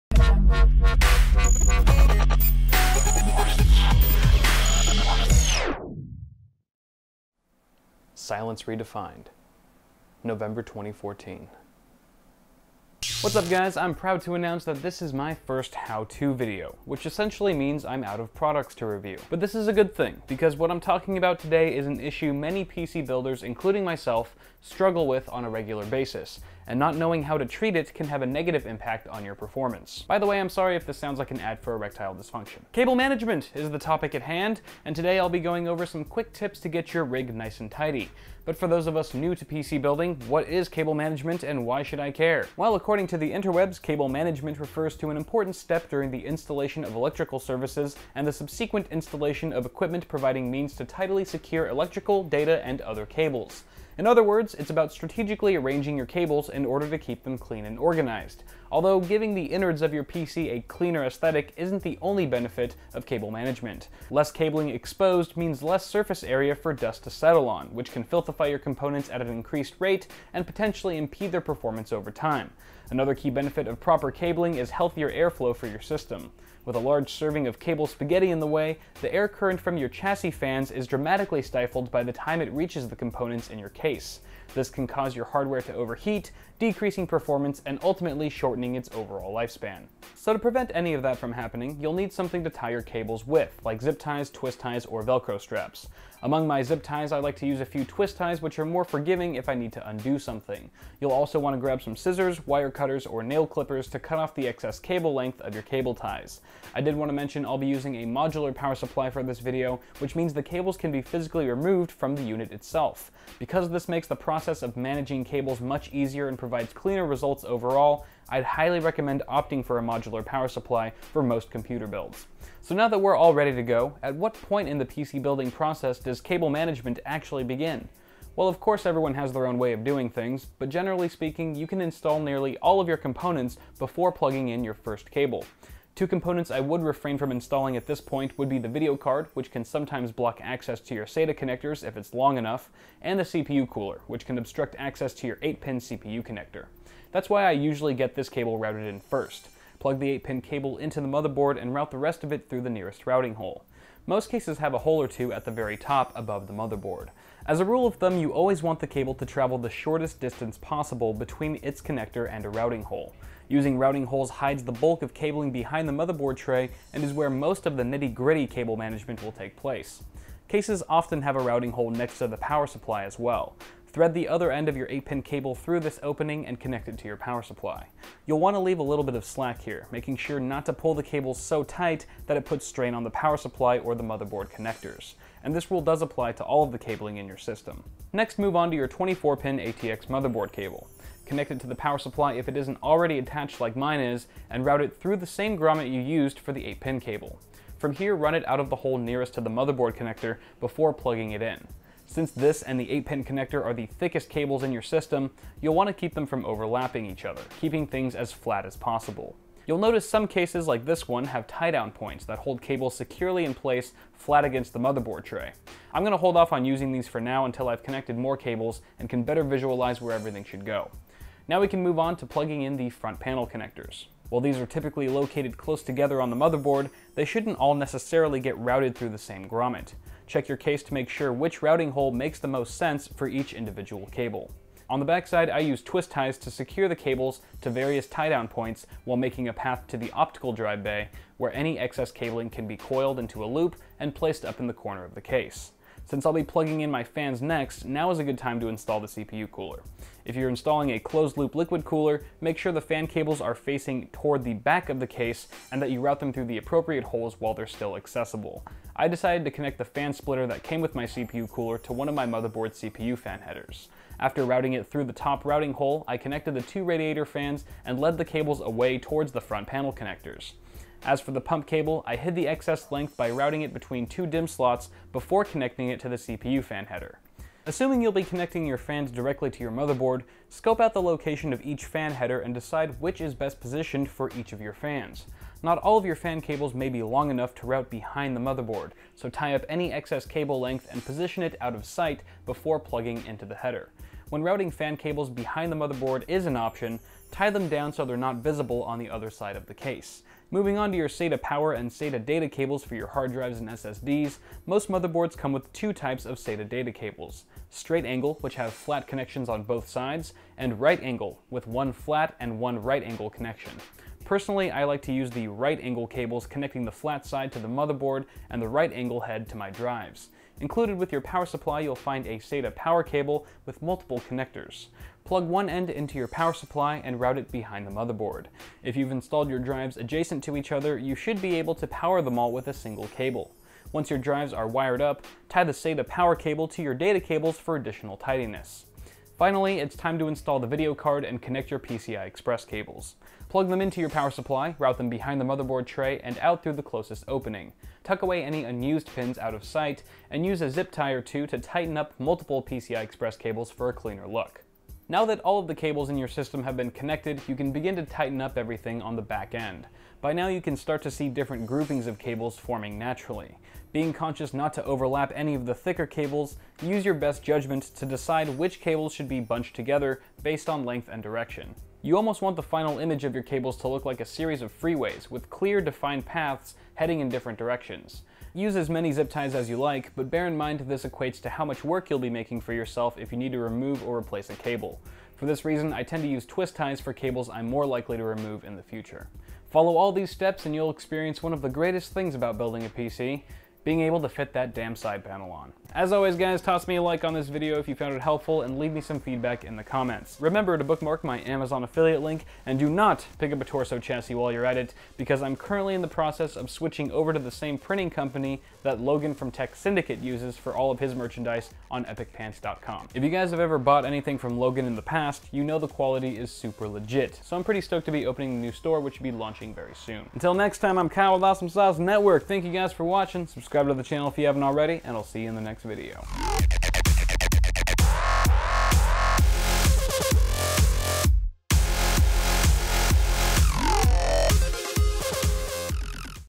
How To: Cable Management
An overview on cable management for your desktop PC.

[G2A Goldmine]

**Parts**
Be Quiet! Dark Power Pro 10 850W
Core i7 5930K
AsRock X99 Extreme4
Corsair Vengeance LPX 16GB (4x4) DDR4 2800
EVGA GTX 970 FTW
Corsair H105
Corsair 750D

✉ SEND FAN MAIL TO:
Bitwit